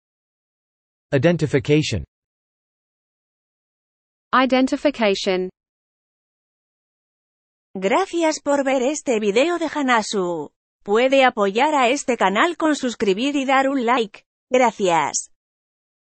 How to pronounce 'Identification' (British English)
How to pronounce 'Identification' in British English.
Comment prononcer 'Identification' en anglais britannique.
Como pronunciar 'Identification' en inglés británico.
Wie man 'Identification' mit britischem Akzent ausspricht.
「Identification」の英語の発音の仕方